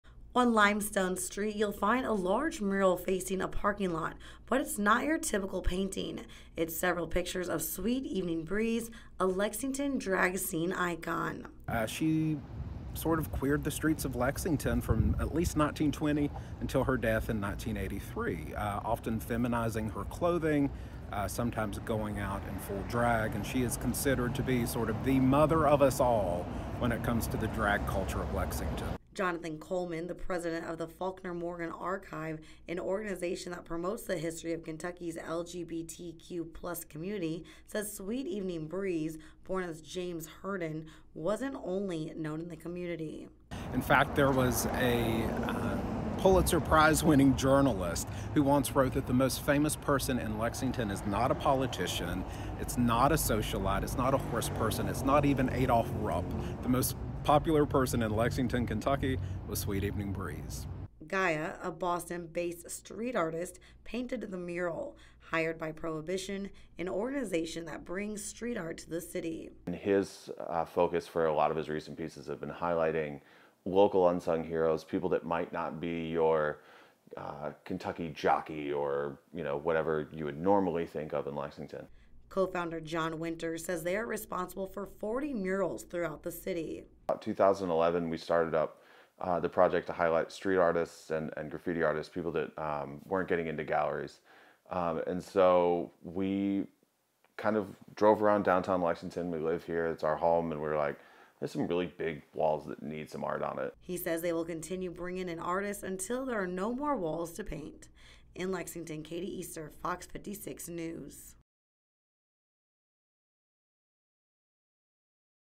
New Mural added to Downtown Lexington
A new mural was added to downtown Lexington. Since 2011 one organization has added 40 murals throughout the city.